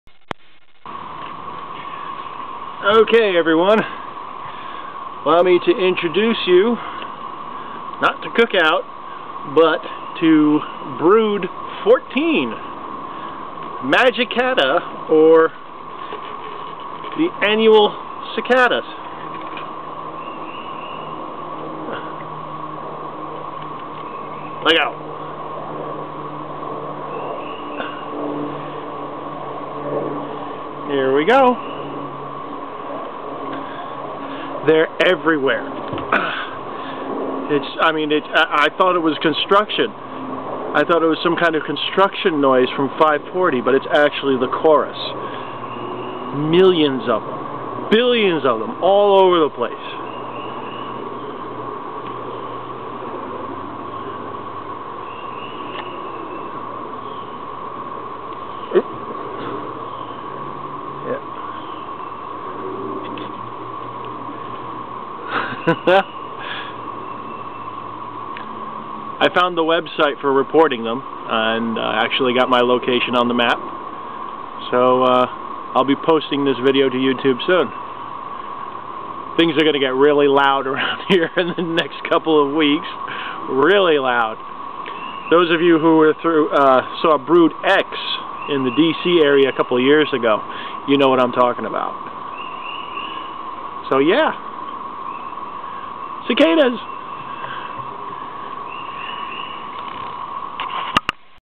Great Southern Brood 2011
Magicicada Brood XIX 2011 - This is a cup full of Magicicada tredecassini.  In the video, I call them "Brood 14", that they're actually Brood Nineteen, also known at the Great Southern Brood.  You can actually hear them in the background on the video!  It sounds like a constant whine, like a soft, jet engine in the distance.  But I'm also hearing M. tredecim in the trees.  The overall noise so far is impressive, but not painful.  YET.  In the next few weeks, it's going to get REALLY LOUD AROUND HERE!  (To my fellow scientists:  apologies for not putting the Latin names in proper italics.  YouTube choked on the HTML for italics.)